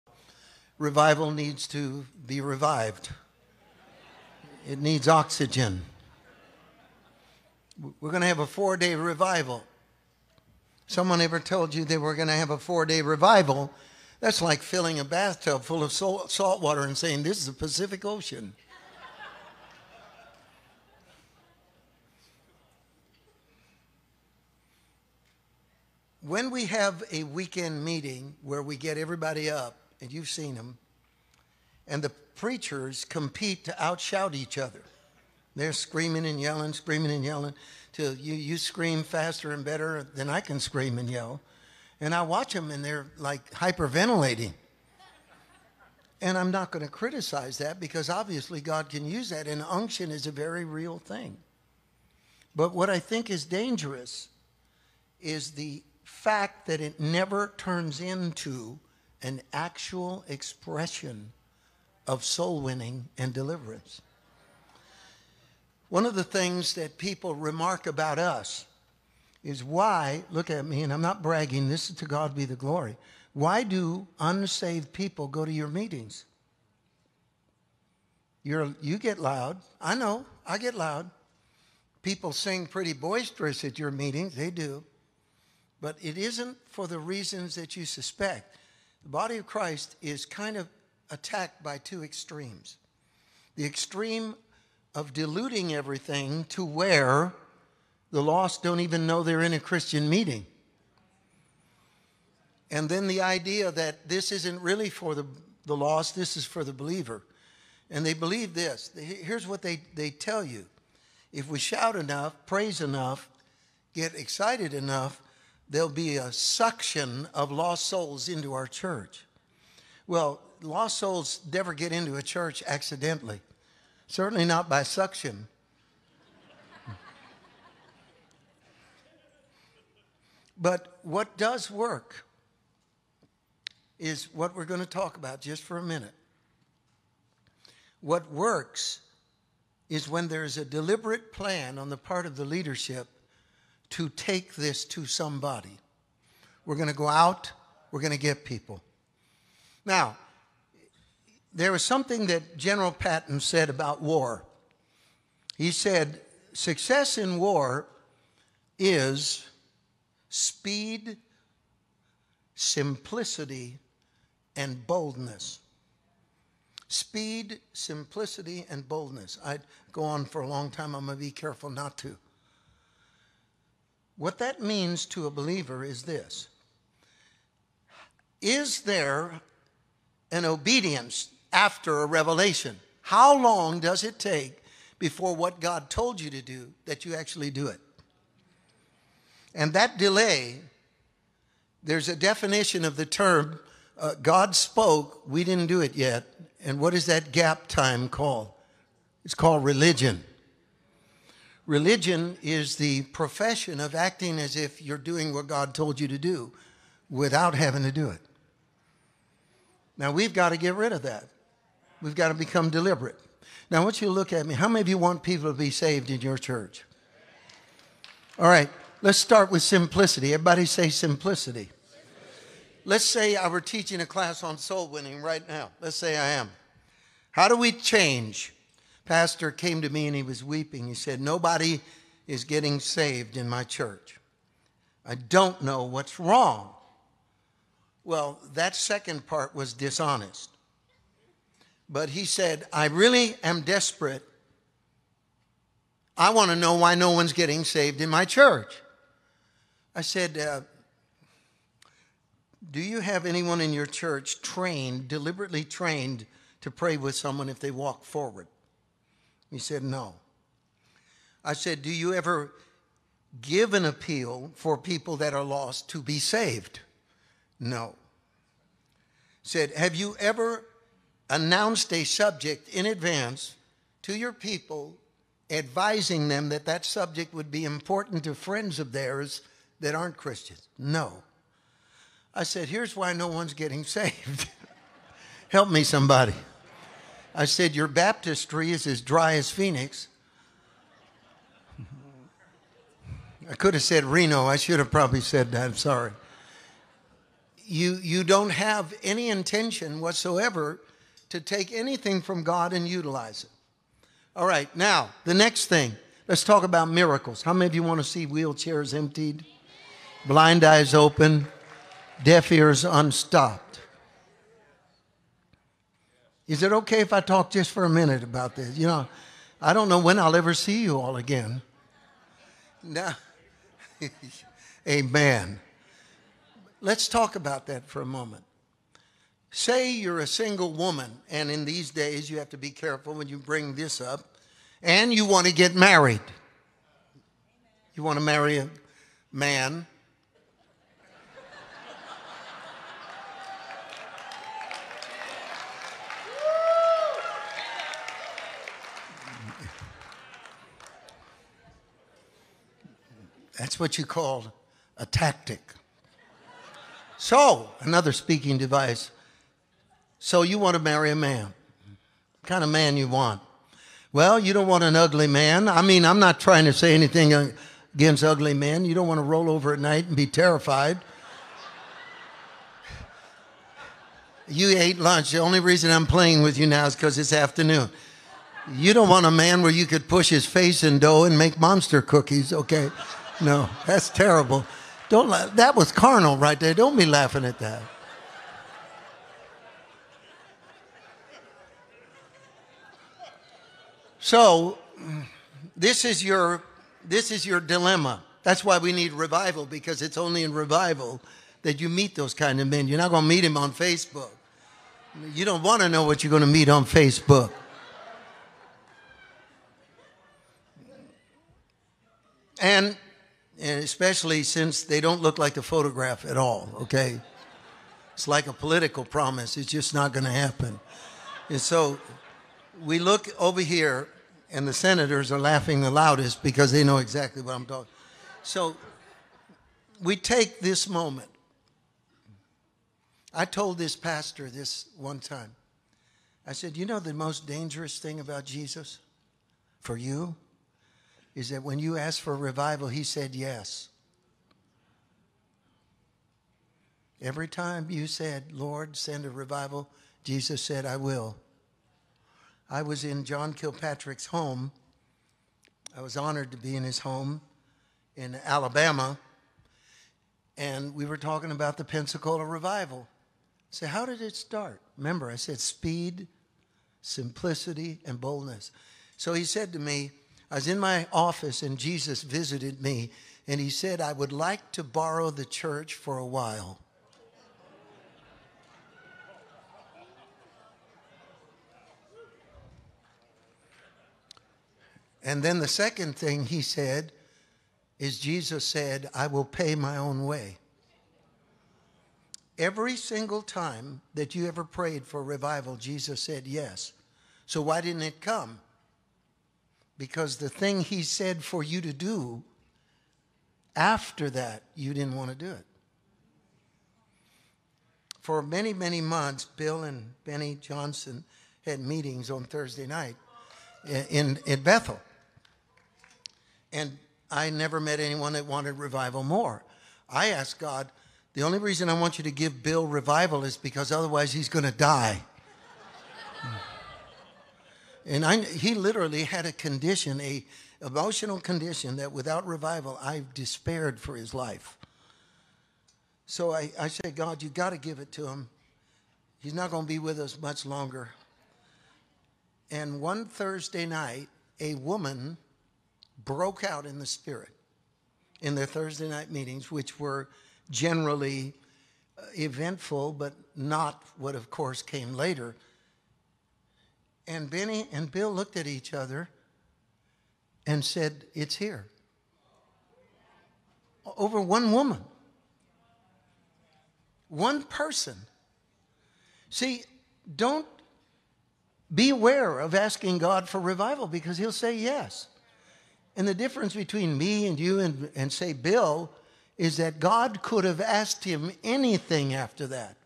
Mario Murillo: Religion is Rebellion Disguised as Obedience (James 4:2)
Mario speaks at a Central California regional Pastor’s lunch on March 4, 2021. Quotes: “Is there an obedience after a revelation? How long does it take before what God told you to do that you do it? God spoke. We didn't do it yet. And what is that gap time called? It's called religion. Religion is the profession of acting as if you're doing what God told you to do without having to do it.

Be blessed.

One of the things that people remark about us is, “Why do unsaved people go to your meetings?” You get loud? I know I get loud. People sing pretty boisterous at your meeting. They do, but it isn't for the reasons that you suspect. The body of Christ is kind of attacked by two extremes. The extreme of diluting everything to where the lost don't even know they're in a Christian meeting. And then the idea that this isn't really for the lost, this is for the believer. And they believe this.

Here's what they tell you. If we shout enough, praise enough, get excited enough. There'll be a suction of lost souls into our church. Well, lost souls never get into a church accidentally. Certainly not by suction. But what does work is what we're going to talk about just for a minute. What works is when there's a deliberate plan on the part of the leadership to take this to somebody. We're going to go out, we're going to get people.

Now, there was something that General Patton said about war. He said, "Success in war is speed, simplicity, and boldness." Speed, simplicity and boldness. I'd go on for a long time. I'm going to be careful not to. What that means to a believer is this. Is there an obedience after a revelation? How long does it take before what God told you to do that you actually do it? God spoke. We didn't do it yet. And what is that gap time called? It's called religion. Religion is the profession of acting as if you're doing what God told you to do without having to do it.

Now we've got to get rid of that. We've got to become deliberate. Now I want you to look at me.
How many of you want people to be saved in your church? All right, let's start with simplicity. Let's say I were teaching a class on soul-winning right now. How do we change? A Pastor came to me and he was weeping. He said, nobody is getting saved in my church. I don't know what's wrong. Well, that second part was dishonest. But he said, I really am desperate. I want to know why no one's getting saved in my church. I said, do you have anyone in your church trained, deliberately trained to pray with someone if they walk forward. He said no. I said, do you ever give an appeal for people that are lost to be saved?

No. I said, have you ever announced a subject in advance to your people, advising them that that subject would be important to friends of theirs that aren't Christians? No. I said, “Here is why no one is getting saved. Your baptistry is as dry as Phoenix. You don't have any intention whatsoever to take anything from God and utilize it.” I said, “Do you know the most dangerous thing about Jesus? It is that when you asked for a revival, and he said, yes. Every time you said, Lord send a revival, Jesus said, I will.”

I was in John Kilpatrick's home. I was honored to be in his home in Alabama. We were talking about the Pensacola revival. I said, "How did it start?" Remember, I said speed, simplicity, and boldness. So he said to me, I was in my office and Jesus visited me. He said, "I would like to borrow the church for a while." Then the second thing he said Jesus said, “I will pay my own way.”

Every single time that you ever prayed for revival. Jesus said, yes. So why didn't it come? Because the thing he said for you to do after that, you didn't want to do it. For many, many months, Bill and Beni Johnson had meetings on Thursday night in Bethel. And I never met anyone that wanted revival more. I asked God, the only reason I want you to give Bill revival is because otherwise he's going to die.

And he literally had a condition, an emotional condition that without revival, I despaired for his life.
So I say, "God, you got to give it to him. He's not going to be with us much longer." And one Thursday night, a woman broke out in the spirit, in their Thursday night meetings, which were generally eventful, but not what, of course, came later. And Beni and Bill looked at each other and said, "It's here." Over one woman. Be aware of asking God for revival because he'll say yes. And the difference between me and you and say, Bill, is that God could have asked him anything after that.